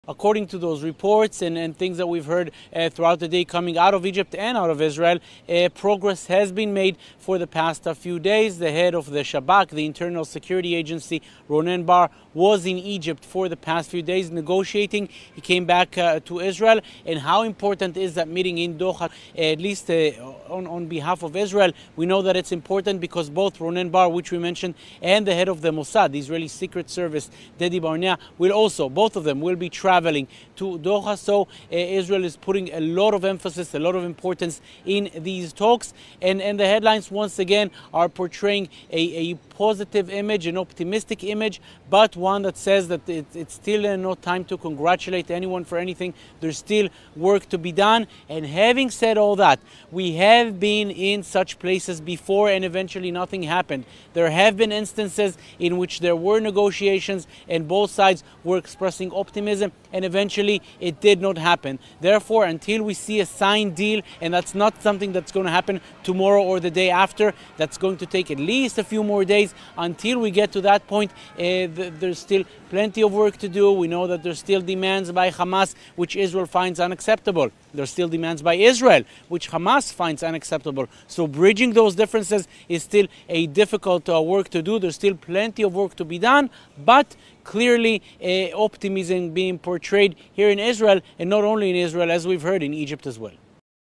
Egypt, US and Israel intelligence chiefs to meet for truce talks in Doha
Efforts to reach a truce will continue today, with the intelligence chiefs of Egypt, the United States, and Israel meeting in Doha. This follows two days of intensive ceasefire negotiations in Cairo. However, Hamas warns that the increased bombardments could jeopardize the fragile truce talks.